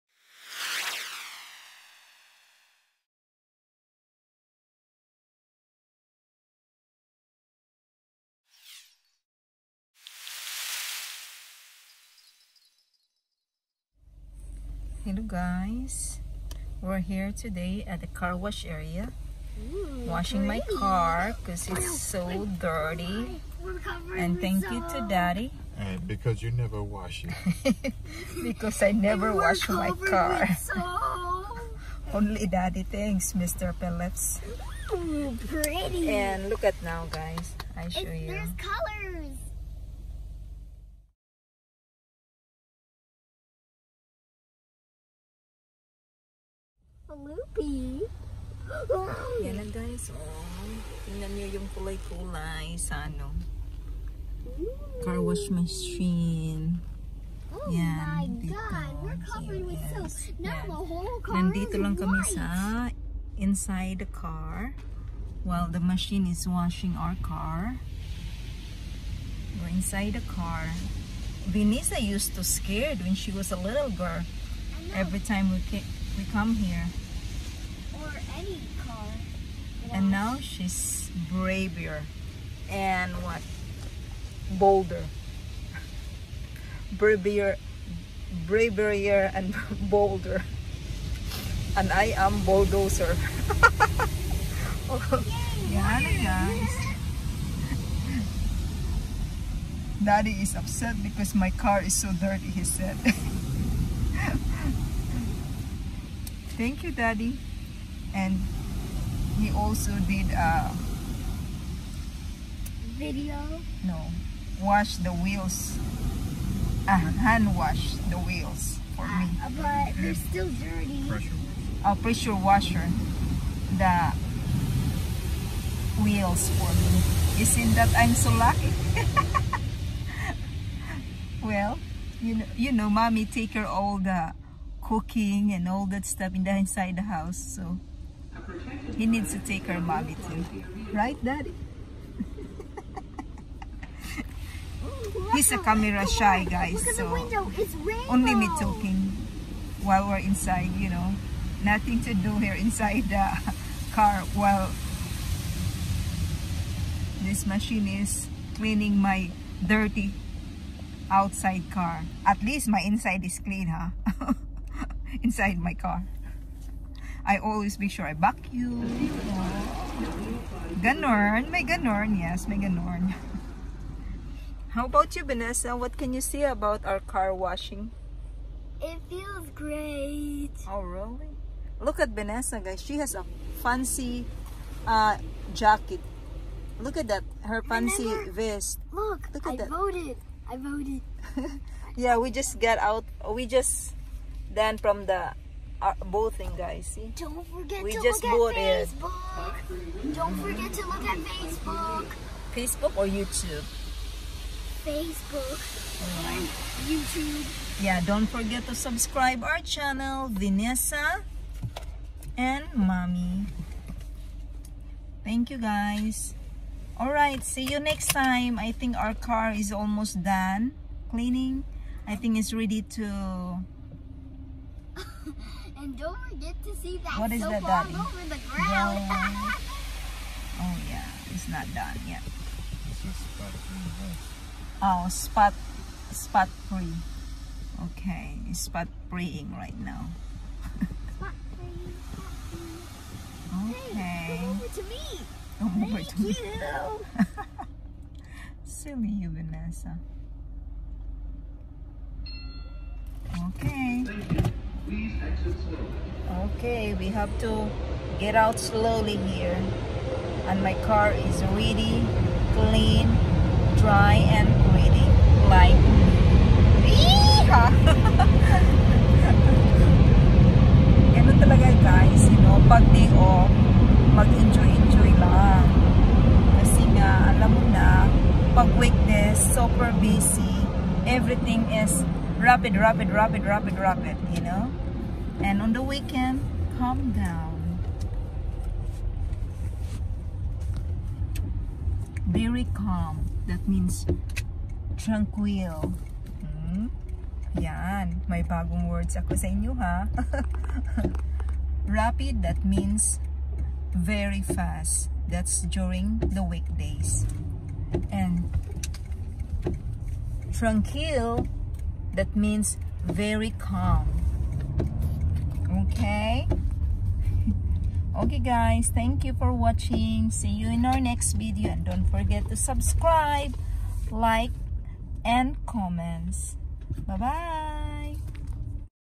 Car wash and silly moments while waiting inside the car..
Husband power  wash my car wheels and ang family silly day inside the car while waiting..